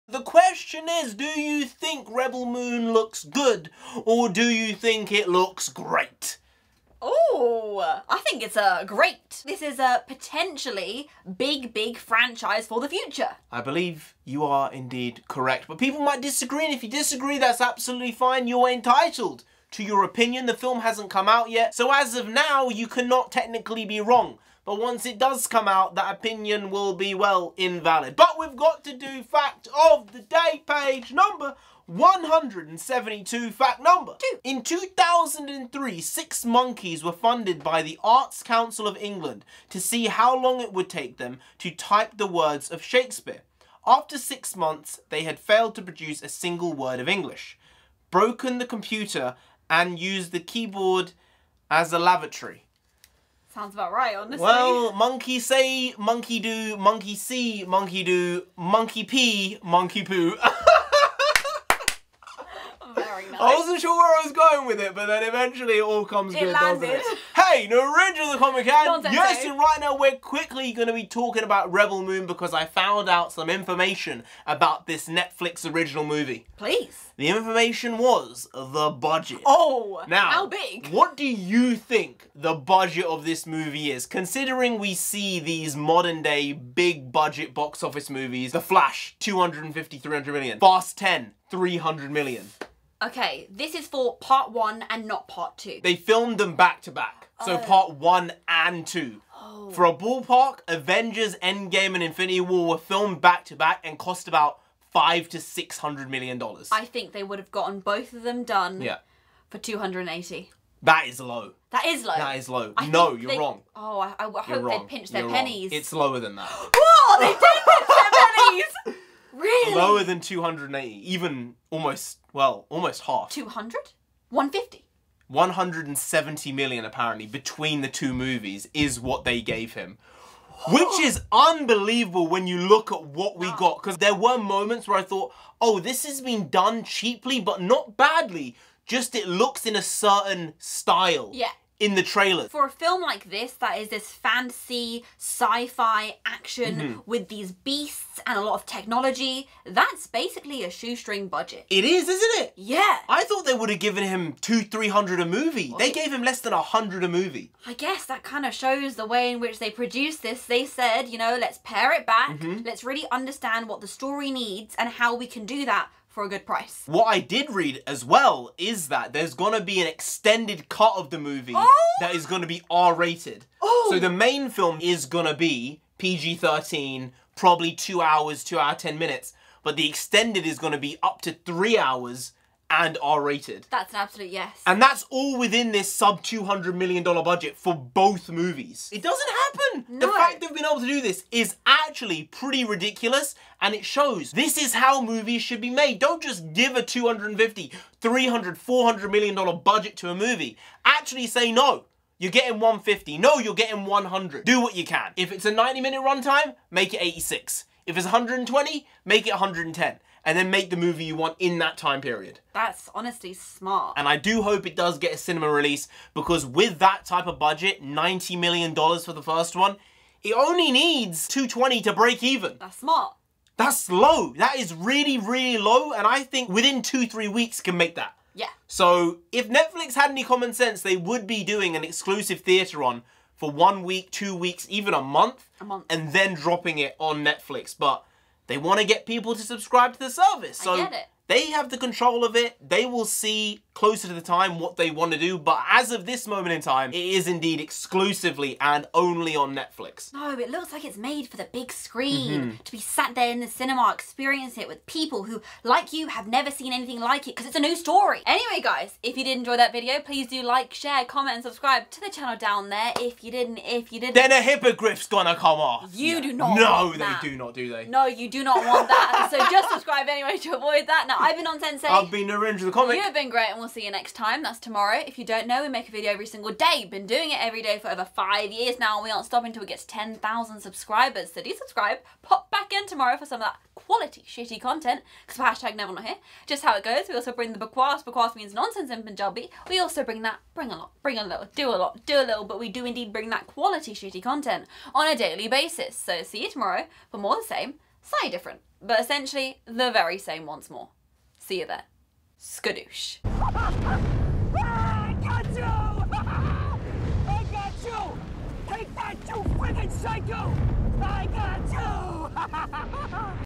Rebel Moon Cost Less Than $100M!!
If the announced budgets of these movies are correct, it's pretty incredible.